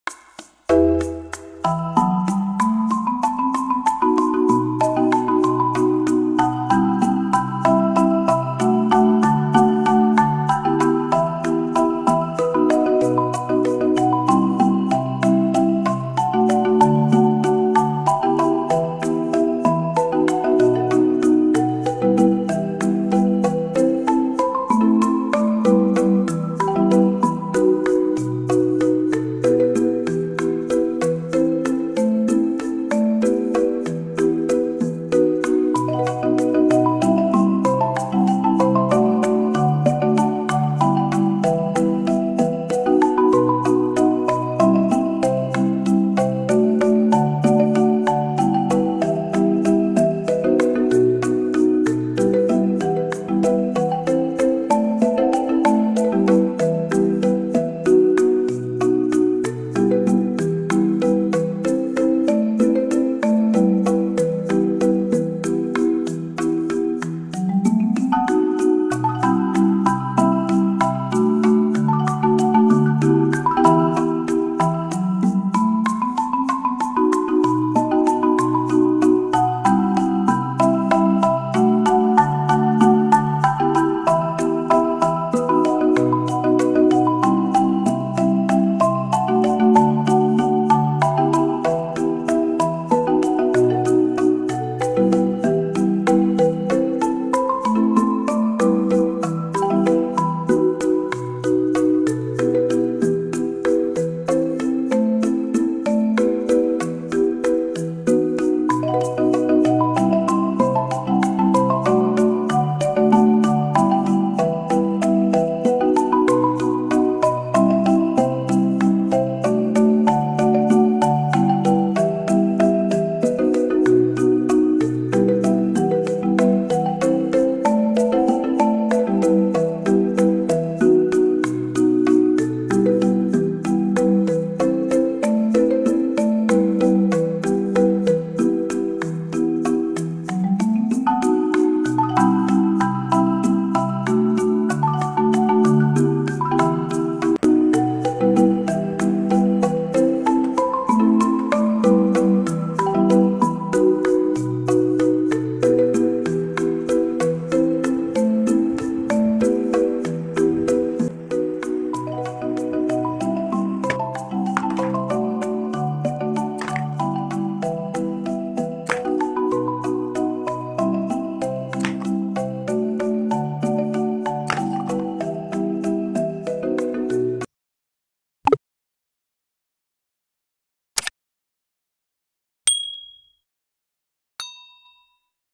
How To Make a Bow and Arrow At Home / Diy bow and Arrow // Homemade Powerfull arrow and bow
Diy homemade simple bow and Arrow .Today i will show you how to make a mini bow & arrow using icecream Stick with easily at your home u make Now

How To Make a Bow and Arrow At Home
Diy bow and Arrow
Homemade Powerfull arrow and bow